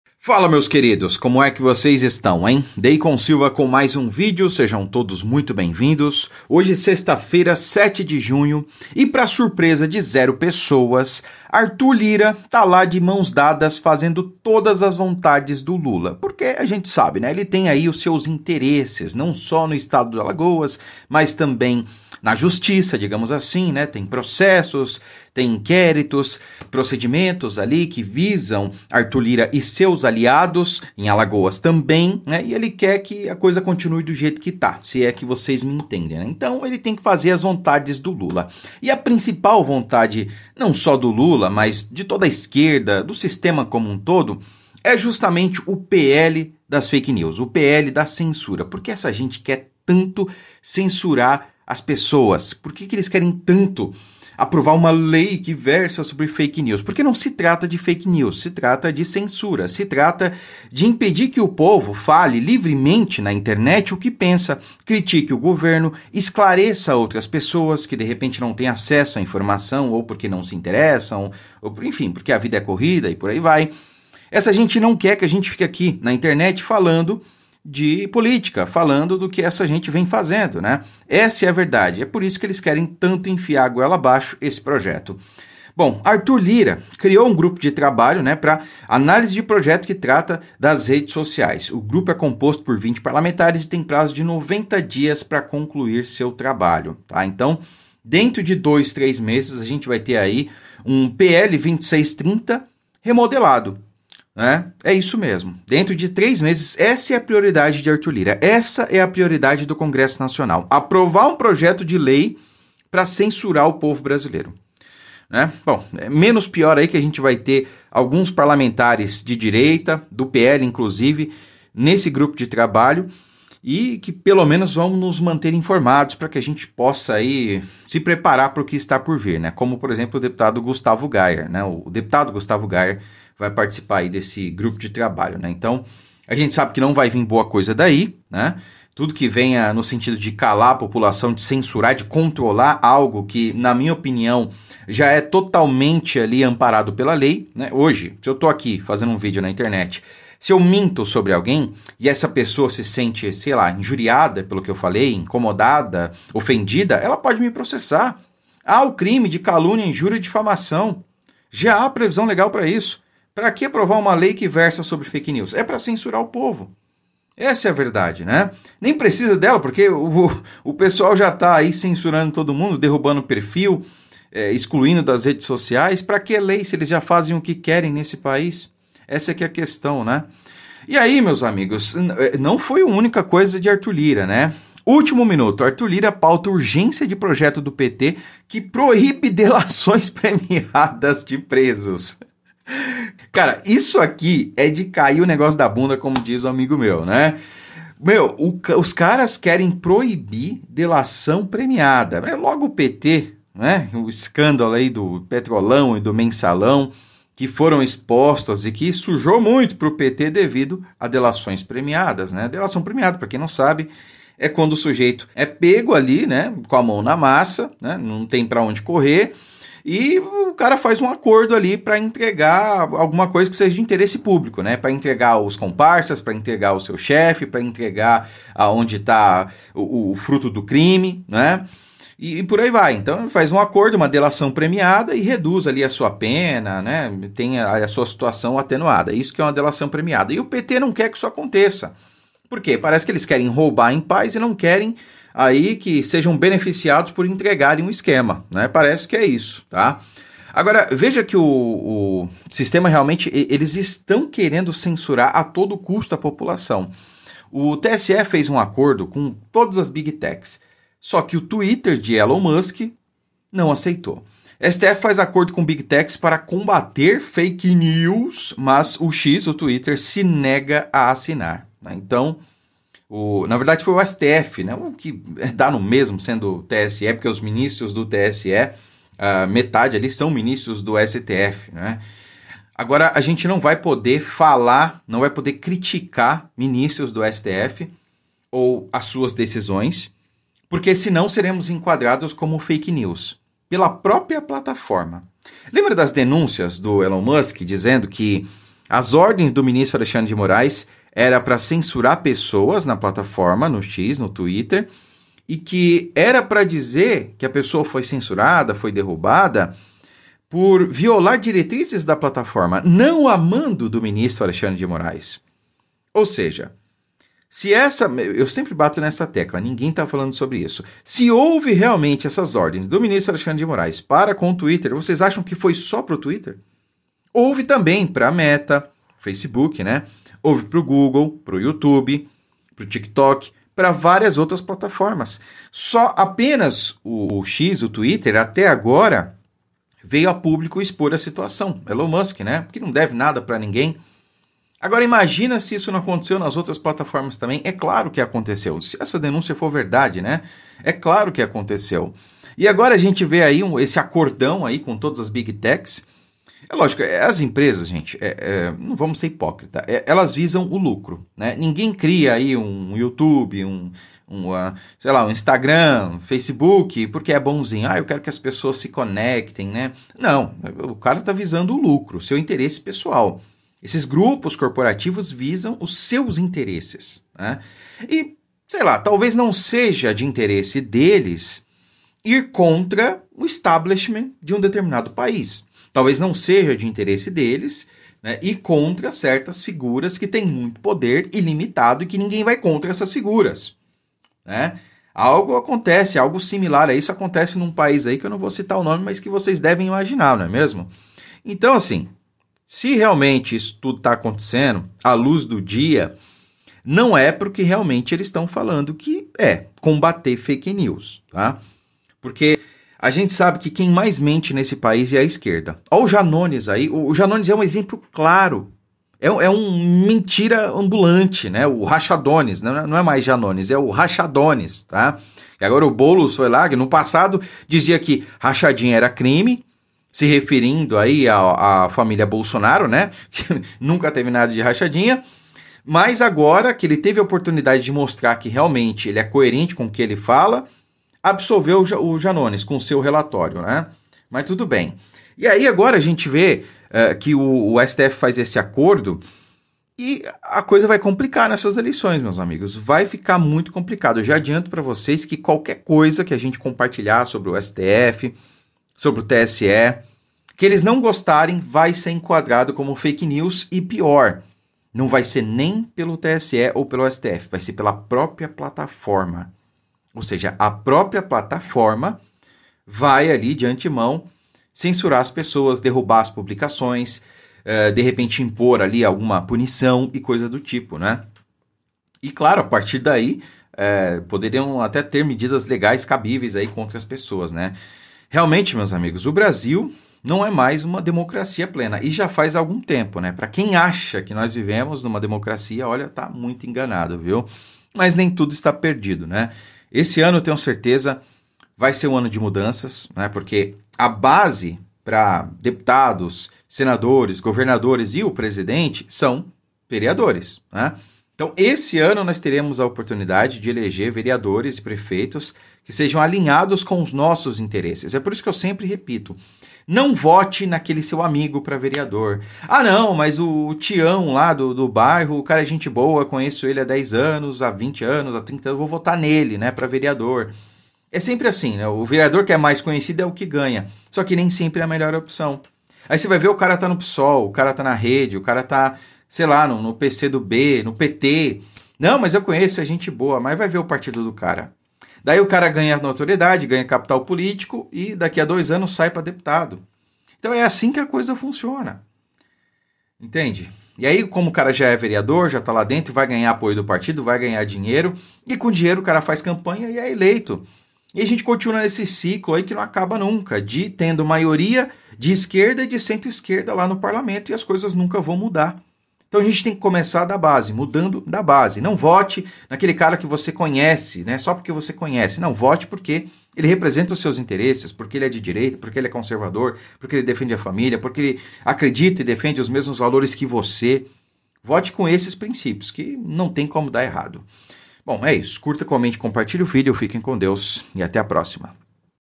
Sistema Avança, Musk dobra aposta e Lira contra o Brasil
O STF fechou acordo com todas as big tech, mas Elon Musk dobrou a aposta e se negou a assinar.
Gostou do conteúdo e quer me ajudar neste trabalho pelo Brasil? Faça uma doação de qualquer valor pelo Pix: